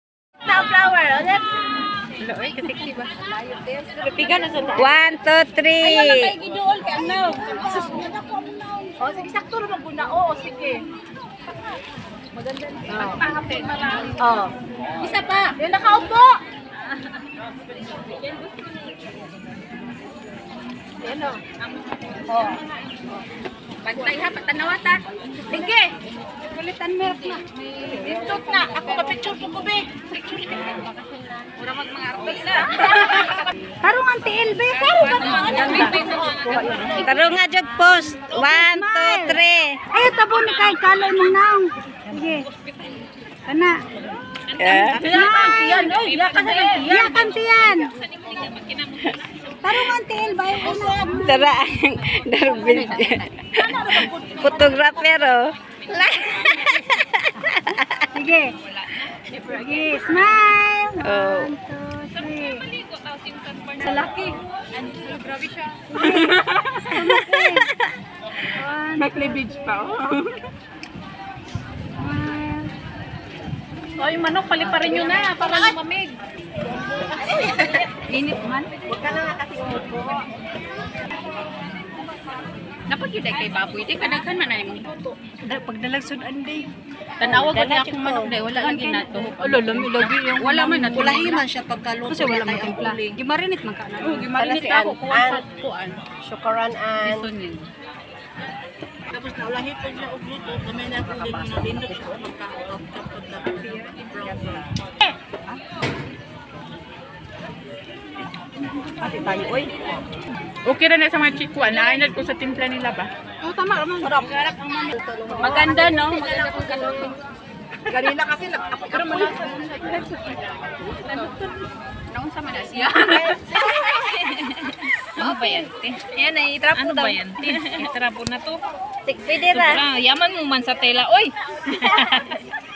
Bonding time and birthday party shekobeach hongkong
bonding time with friends at shek o beach Hong Kong